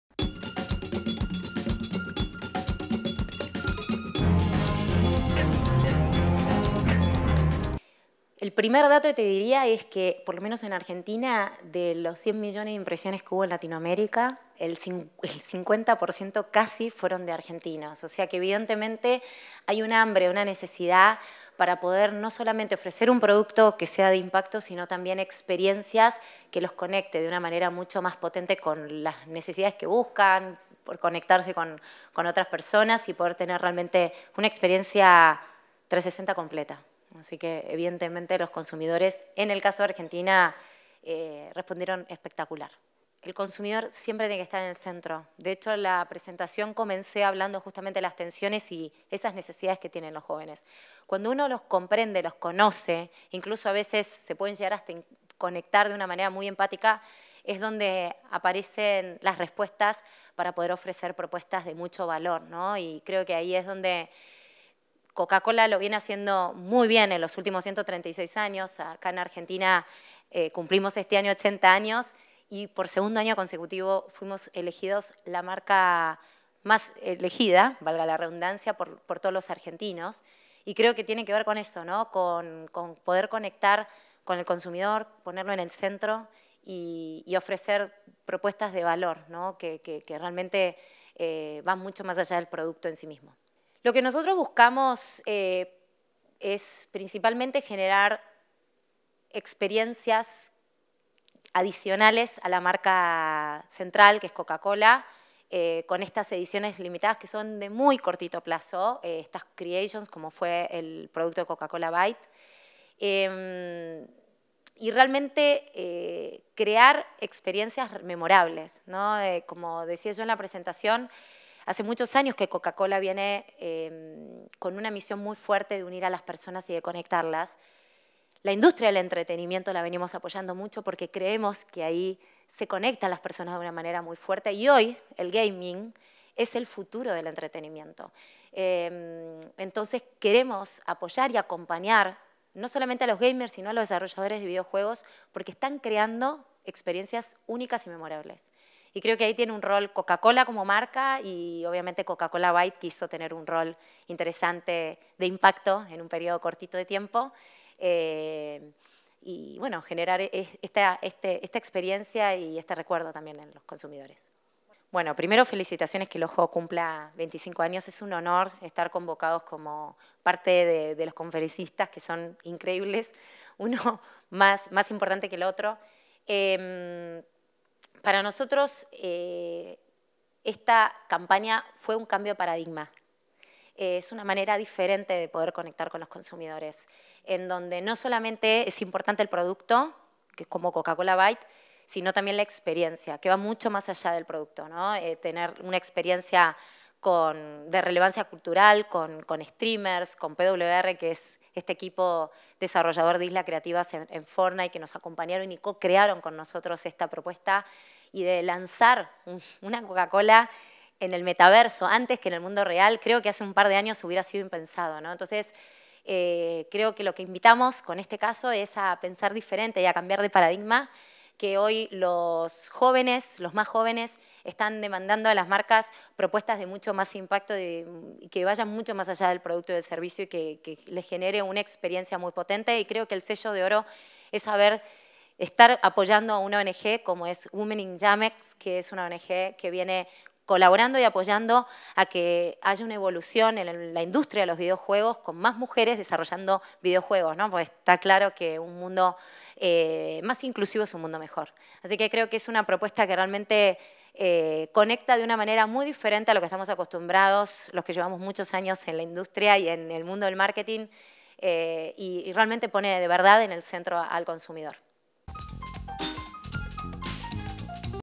Denise Picot – Entrevista El Ojo 2022
En su paso por El Ojo de Iberoamérica, Denise Picot, Directora de Marketing de Argentina y de Uruguay en Coca Cola Company, analizó el caso Coca Cola Creation, a continuación brindo una entrevista al equipo de LatinSpots.